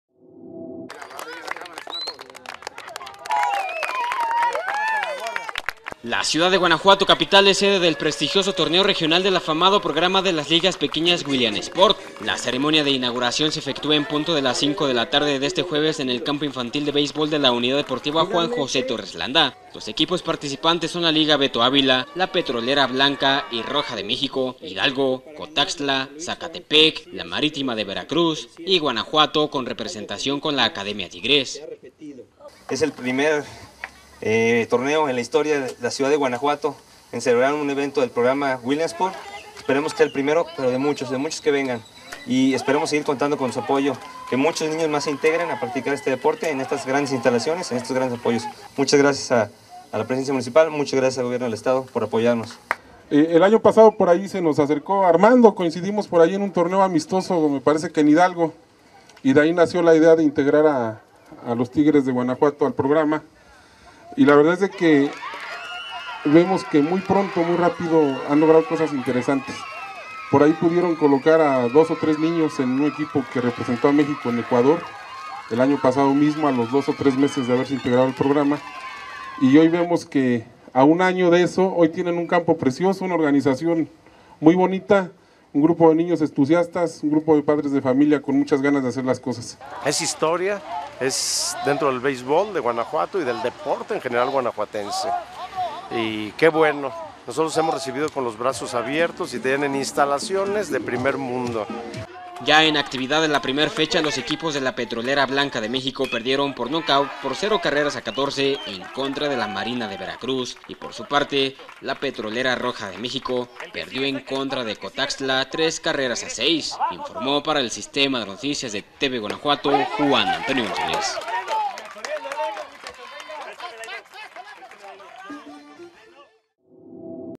GUANAJUATO ORGANIZA TORNEO REGIONAL WILLIAM SPORT
Por primera vez Guanajuato es sede de un evento de tal relevancia en el beisbol infantil y se anunciaba que entre otros equipos venían ya clasificados de su etapa distrital, de las ligas de Cotaxtla, Marítima y Beto Ávila de Veracruz, así como Hidalgo, Zacatepec y los Tigres de Guanajuato.